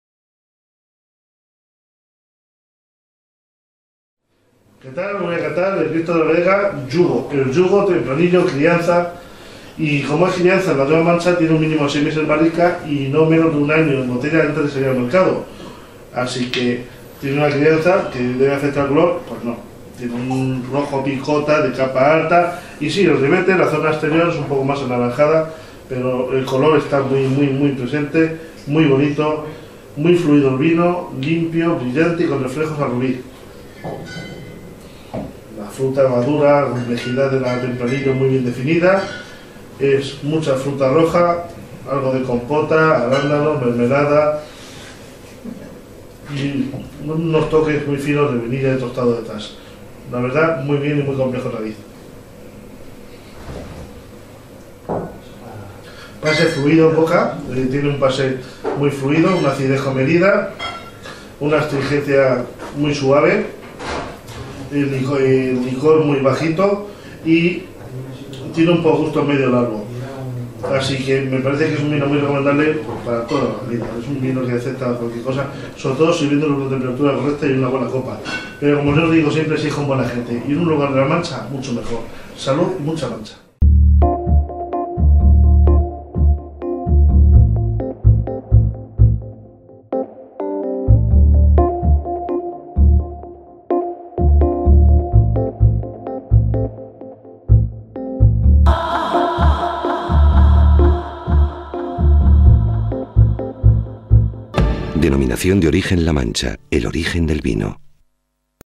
Videocata de El Yugo tempranillo crianza
Cata del Tempranillo crianza El Yugo, elaborado por Bodegas Crisve, Cristo de la Vega de Socuellamos, provincia de Ciudad Real, amparado en el marco de la denominación de origen La Mancha.
Sumiller Adán Israel.